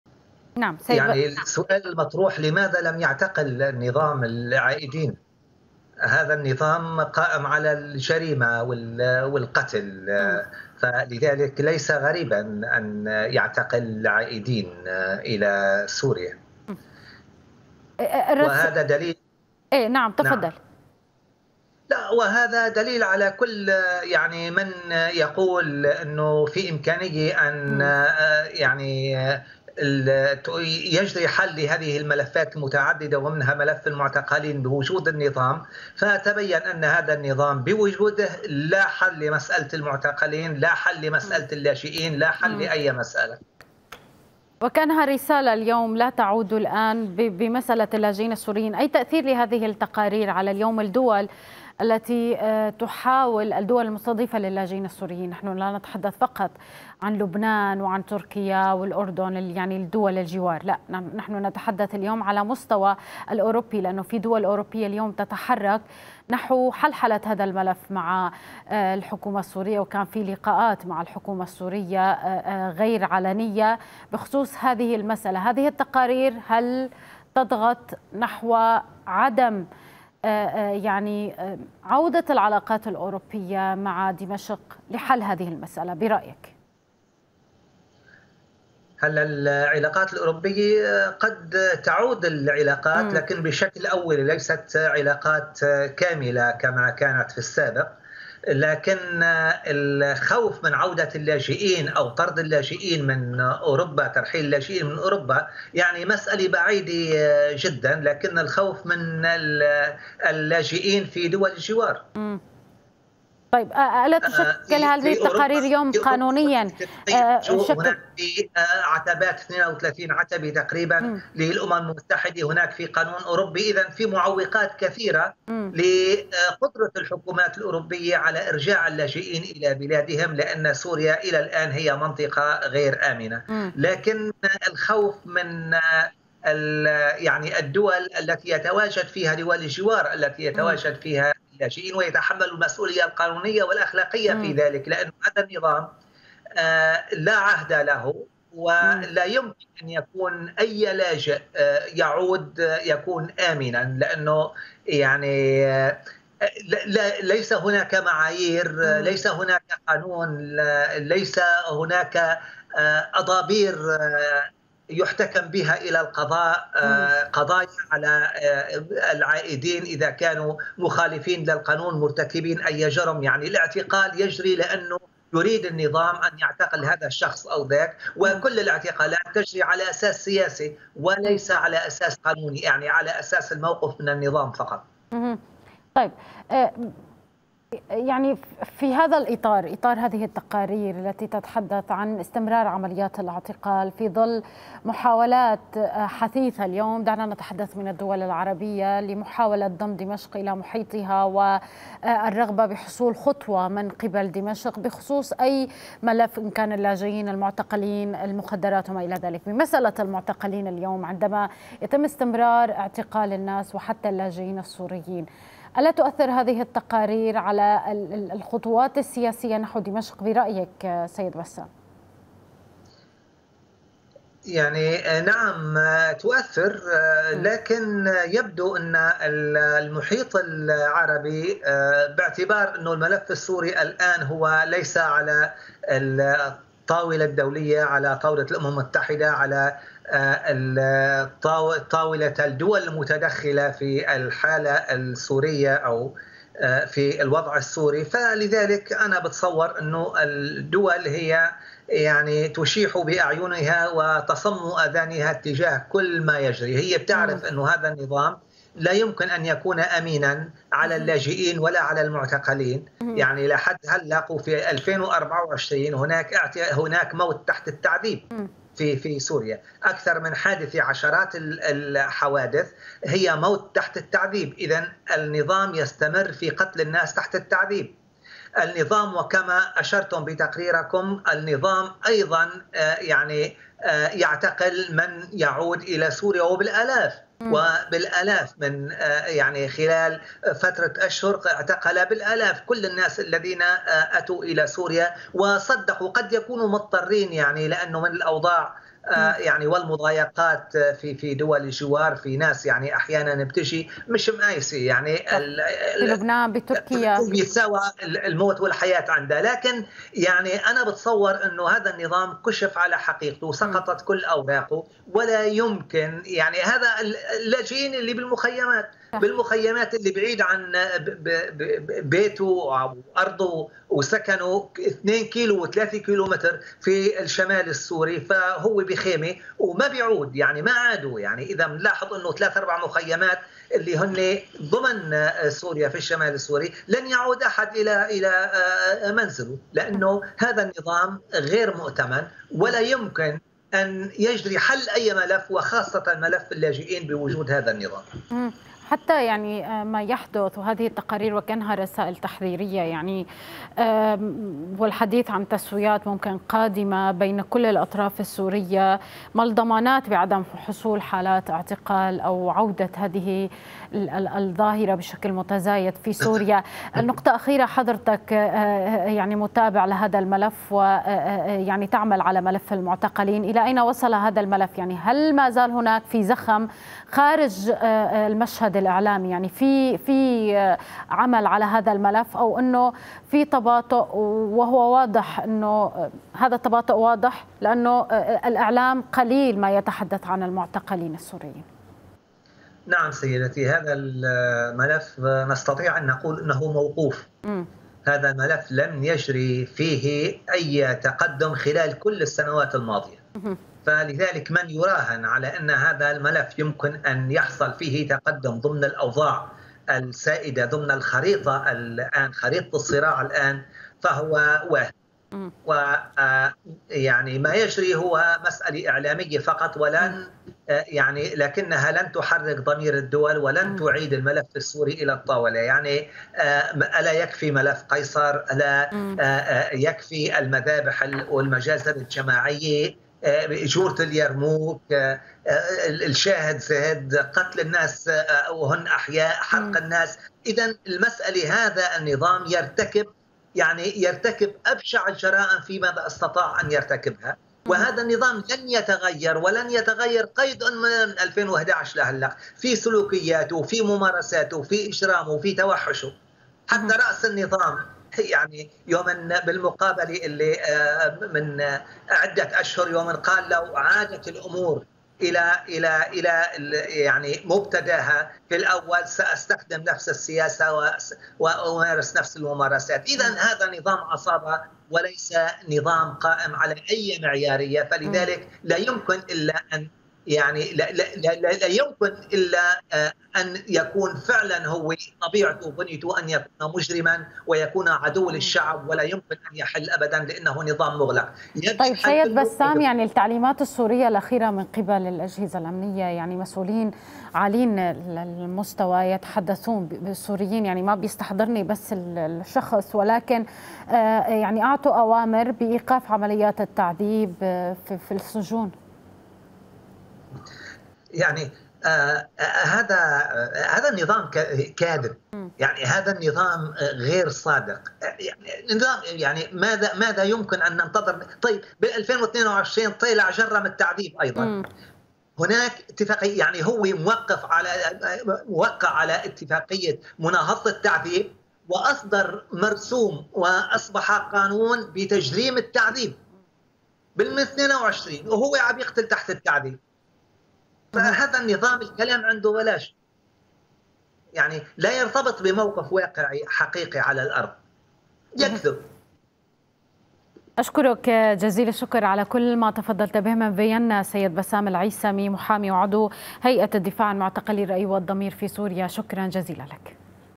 بسام العيسمي: من يراهن على ملف المعتقلين في إحداث تقدم في ظل هذه الأوضاع فهو واهم
فيينا
بسام العيسمي - محام وعضو هيئة الدفاع عن معتقلي الرأي والضمير في سوريا: من يراهن على ملف المعتقلين في إحداث تقدم في ظل هذه الأوضاع فهو واهم
البث الحي / البث المباشر الأعلى مشاهدة

تابعوا قناة اليوم على التردد:
12687_27500 (H) _NILESAT